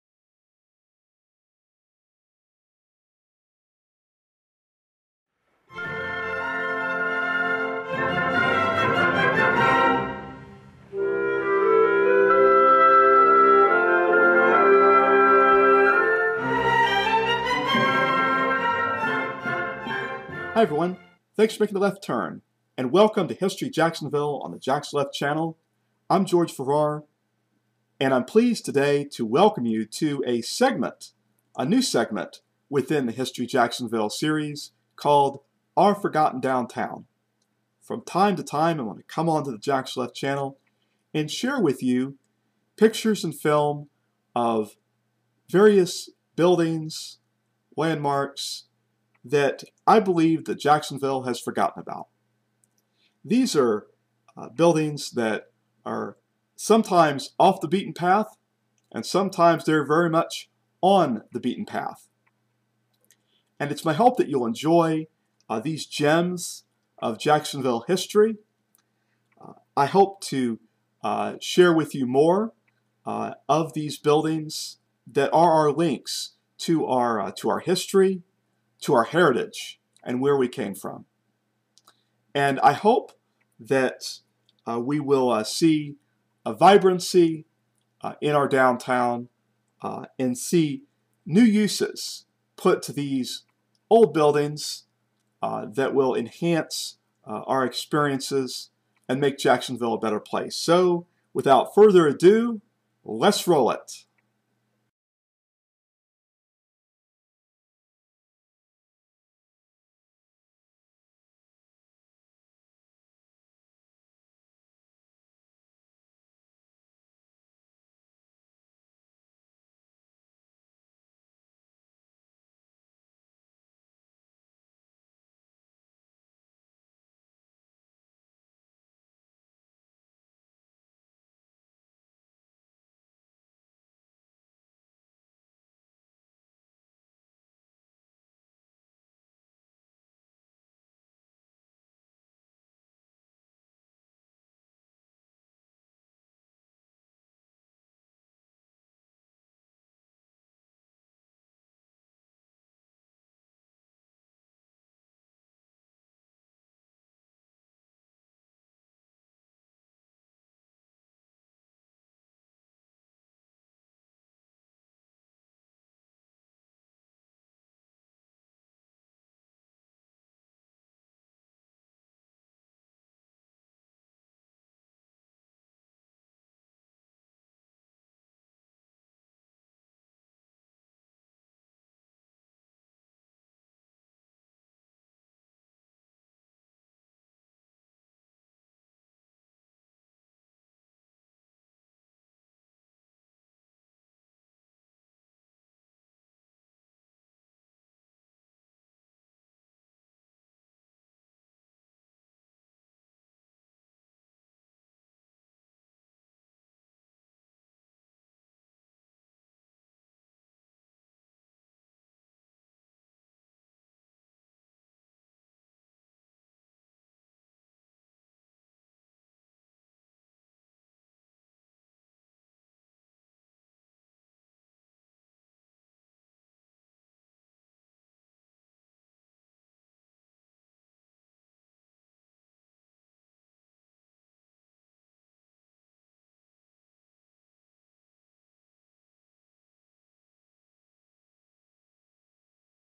History Jacksonville! Our Forgotten Downtown-1920s and 1950s Architecture
Downtown Jacksonville, Jacksonville History, Hogan St, Julia St, Jones Brothers Furniture, Elena Flats Building, Duval St, Ashley St
***HJPP-The History Jacksonville Preservation Project***
After I started Season 2 of History Jacksonville on the-then brand new “TheJaxLeft” Channel on YouTube, I did a flurry of street filming in December, 2013 and January of 2014 which resulted in lots of photos and film that I produced into 7 shows. This show was the one of the 1st of what I call “clip shows” and it started out as an experiment to see if I could effectively introduce some of Jacksonville’s rich architectural history. This format allowed me great freedom to just bring the viewer quietly what I saw without complexity or commentary. The focus in this show was on buildings that had faded into the background over the years in what I called “Our Forgotten Downtown” which became a time to time segment within the HJ series and I later renamed “Our Rediscovered Downtown”. The viewers enjoyed Show # 16 and it spiked in popularity. It ended up being the 21st most popular of the 178 shows on the channel. It got my attention - and as a result -the series grew. The last picture you’ll see in the show is The Elena Flats Building which I would focus on in Show #30 a year later. Years later it would be renovated. This is the 1st Classic. In this show I saw past greatness and sought to present it for consideration for renovation back into the modern city.
George Farrar

Original Description Text (Revised)
Today on History Jacksonville, I welcome you to a new segment within the series called "Our Forgotten Downtown". We explore Jacksonville's architecture from the 1920s thru the 1950s.  Please Check Out for even more!